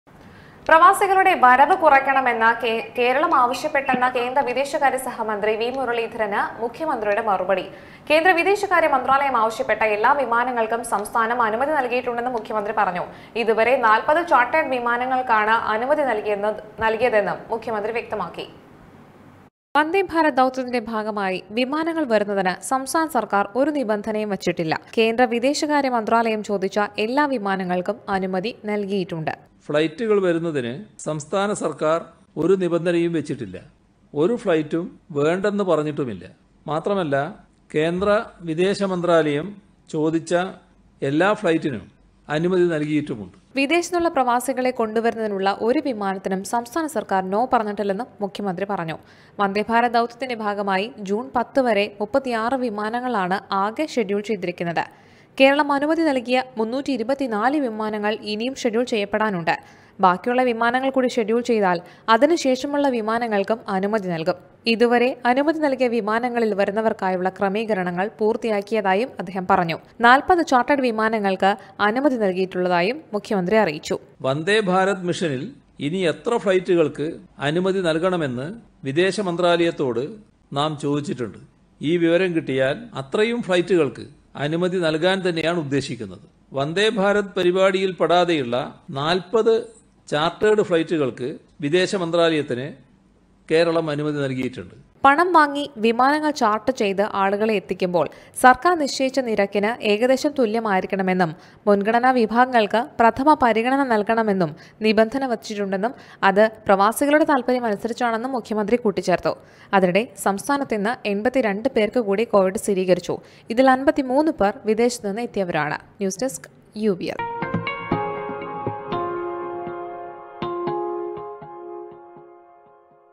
UBL NEWS | 40 ചാര്‍ട്ടേഡ് വിമാനങ്ങള്‍ക്ക് കേരളത്തിന്റെ അനുമതി | 03.06.2020
പ്രവാസികളുടെ വരവ് കുറയ്ക്കണമെന്ന് കേരളം ആവശ്യപ്പെട്ടെന്ന കേന്ദ്ര വിദേശകാര്യസഹമന്ത്രി വി മുരളീധരന് മുഖ്യമന്ത്രിയുടെ മറുപടി. കേന്ദ്ര വിദേശകാര്യമന്ത്രാലയം ആവശ്യപ്പെട്ട എല്ലാ വിമാനങ്ങള്‍ക്കും സംസ്ഥാനം അനുമതി നല്‍കിയിട്ടുണ്ടെന്ന് മുഖ്യമന്ത്രി പറഞ്ഞു.ഇതുവരെ 40 ചാര്‍ട്ടേഡ് വിമാനങ്ങള്‍ക്ക് അനുമതി നല്‍കിയതായും മുഖ്യമന്ത്രി വ്യക്തമാക്കി.